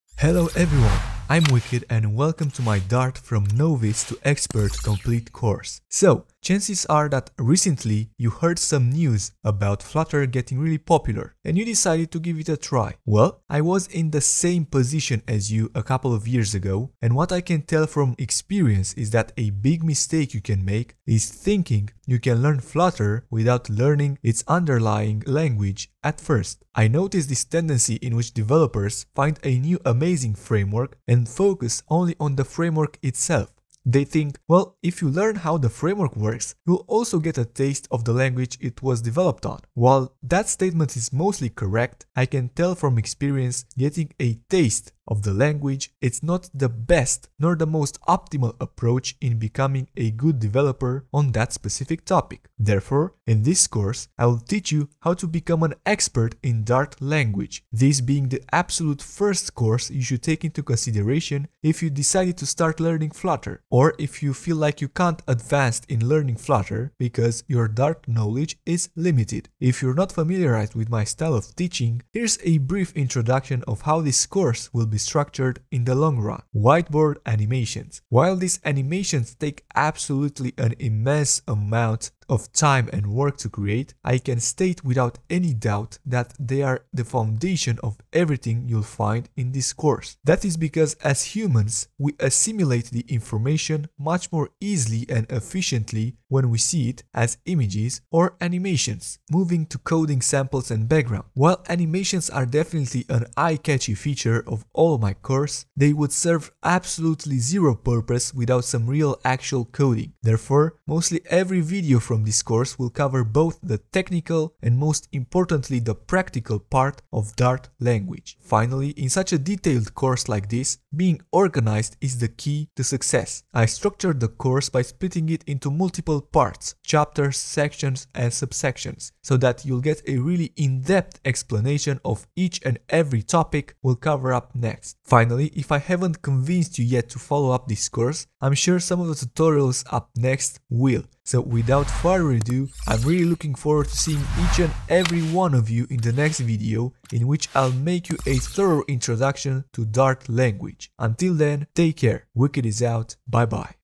Introduction to Dart - From Novice to Expert Tutorial Series
Hi there!

In this short video I'll introduce you to the brand new series in which I'll make you a Dart Expert.

Also, if you want to support me furthermore you can become an YouTube Member, donate or buy my courses on Udemy, using the following links: